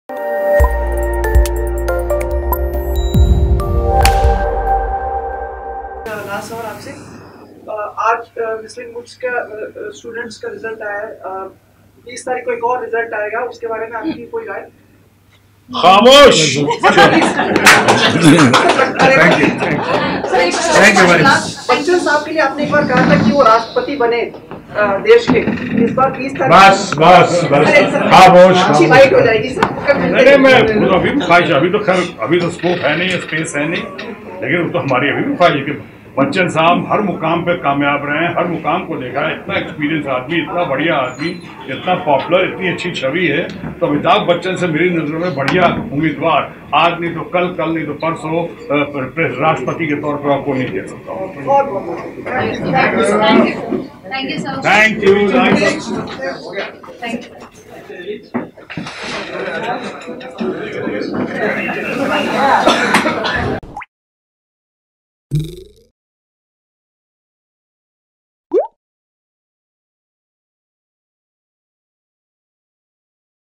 Shatrughan Sinha Reaction On President Election Results 2017 | Funny Answer
Watch Shatrughan Sinha Reaction On President Election Results 2017 | Funny Answer